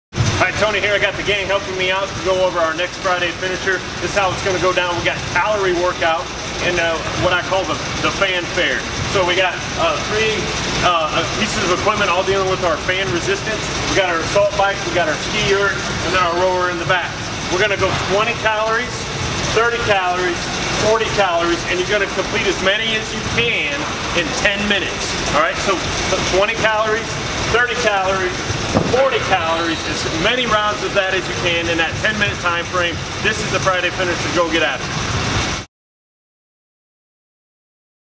Fanfair Friday finisher workout challenge
Friday Finisher workout! Put the fan resistance to work and burn some serious calories!